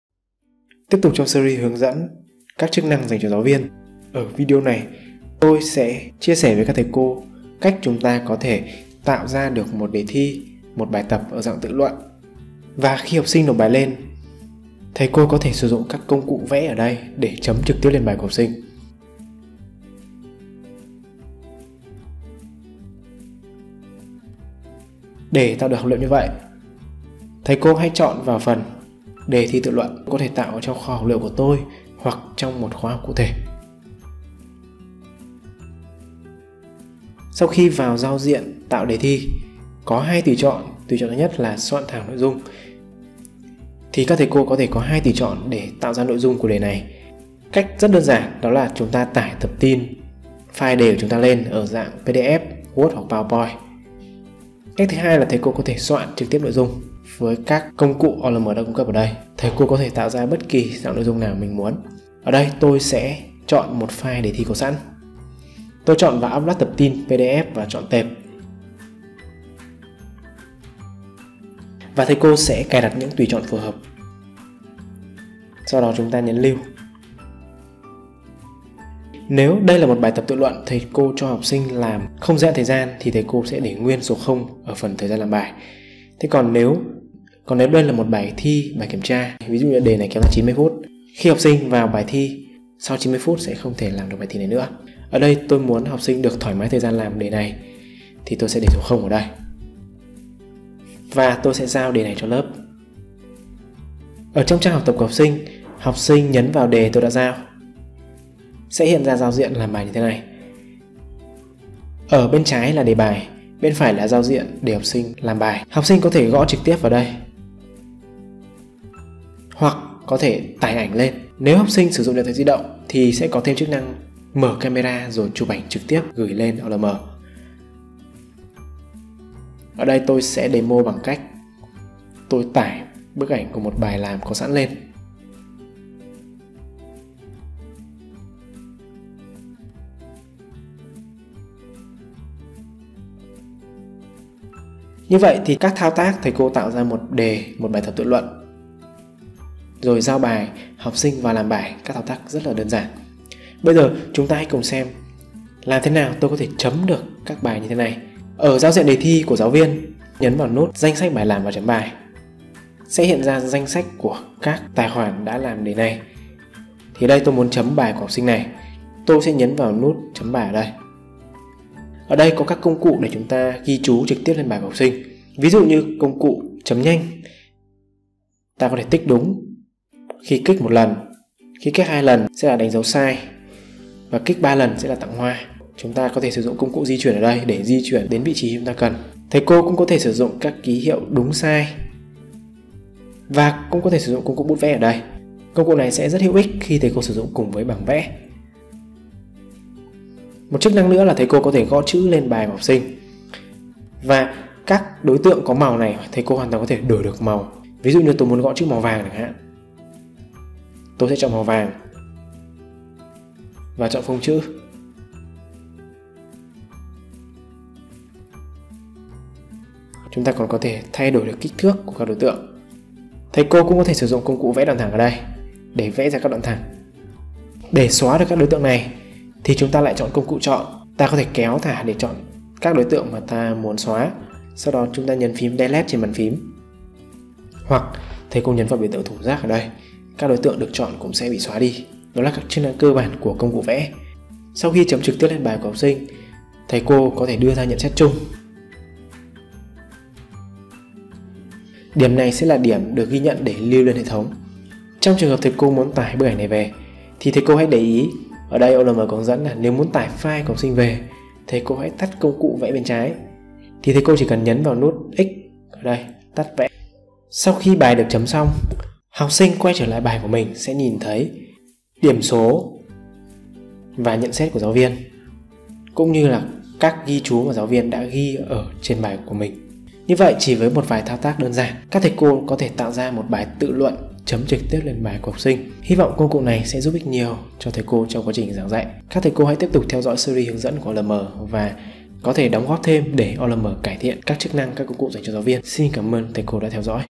Tạo đề tự luận và chấm trực tiếp lên bài làm | OLM.VN
Hướng dẫn tạo đề tự luận chấm trực tiếp lên bài của học sinh.